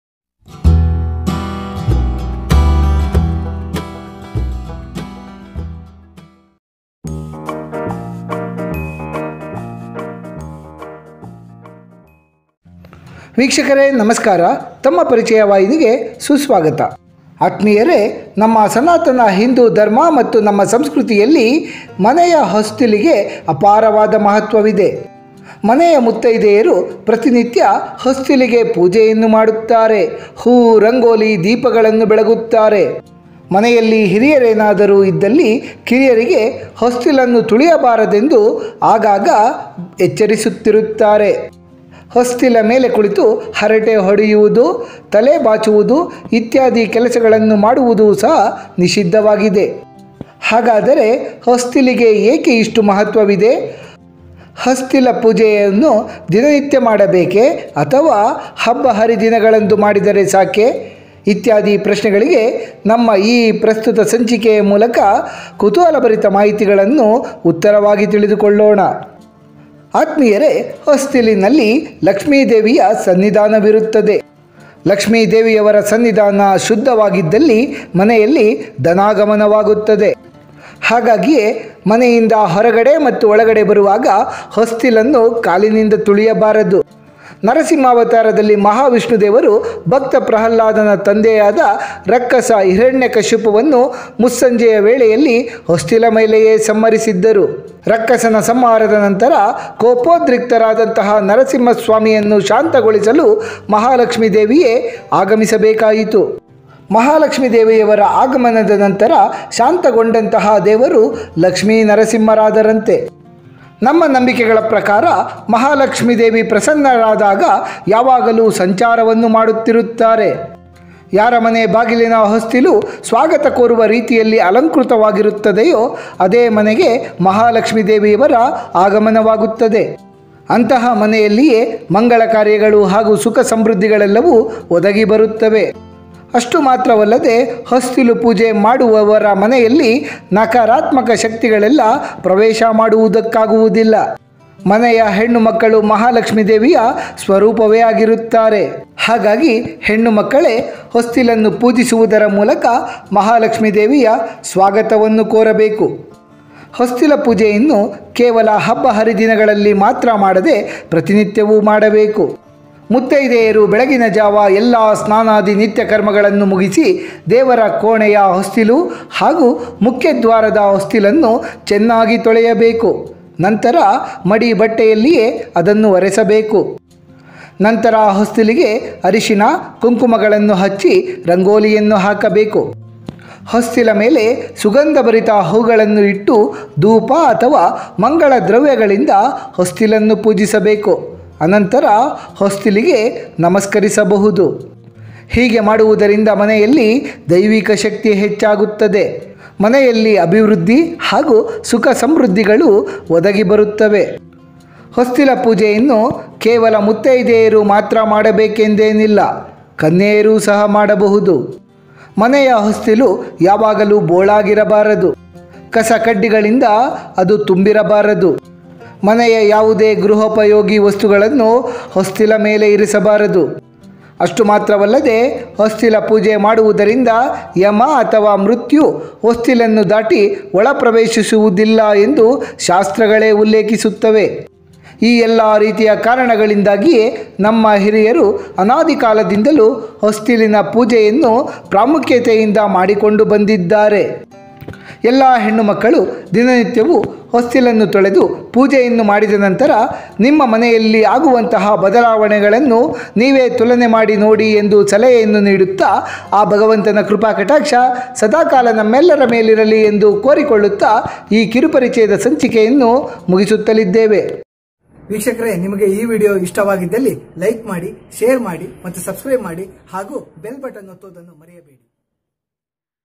ಹೊಸ್ತಿಲು ಪೂಜೆಯನ್ನು ಹೇಗೆ ಮಾಡಬೇಕು ? ಯಾಕೆ ಮಾಡಬೇಕು ? Threshold pooja
ಹೊಸ್ತಿಲು ಪೂಜೆಯ ಮಹತ್ವ
Importance of threshold pooja